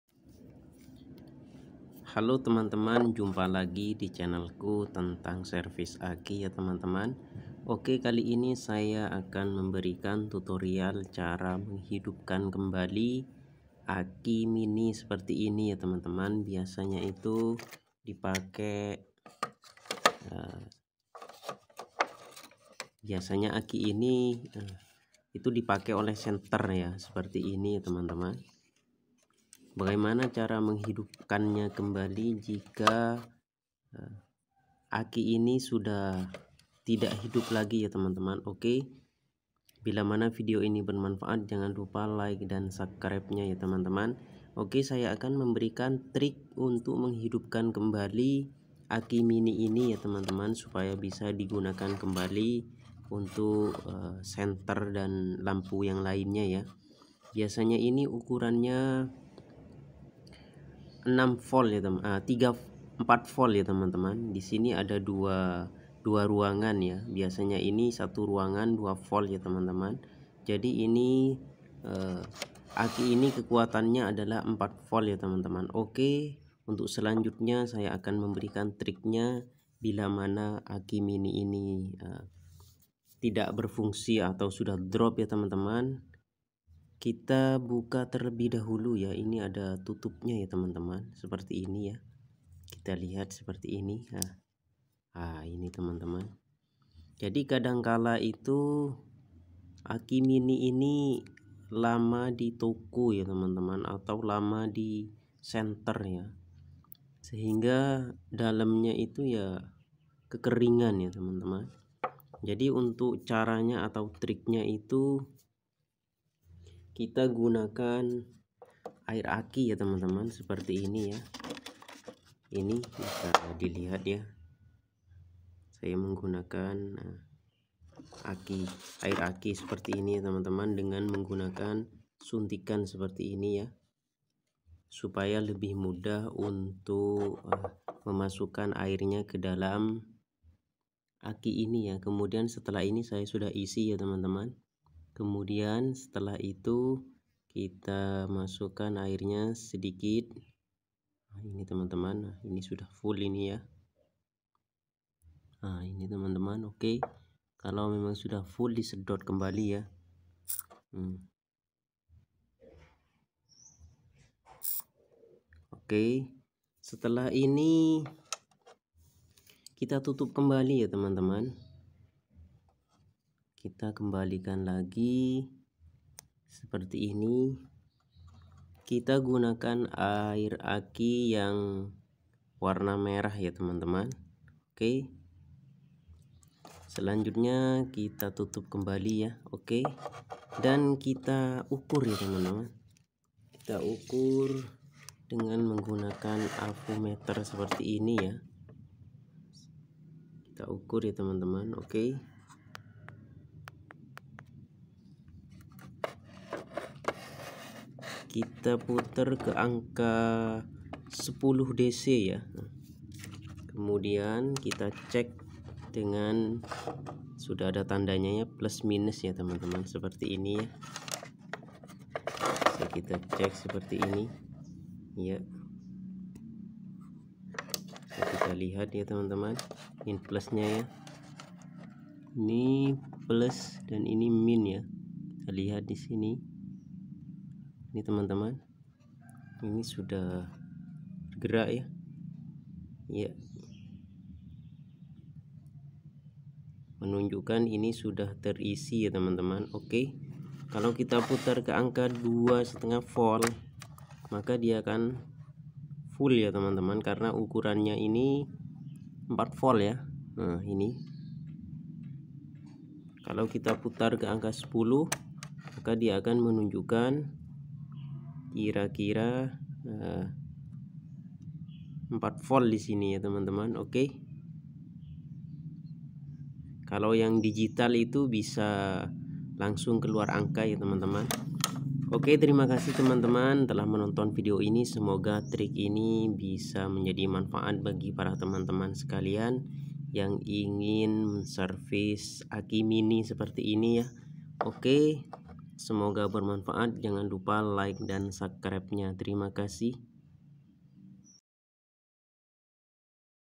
trik menghidupkan kembali aki senter yang rusak
tutorial menghidupkan kembali aki senter yang rusak supaya bisa di gunakan kembali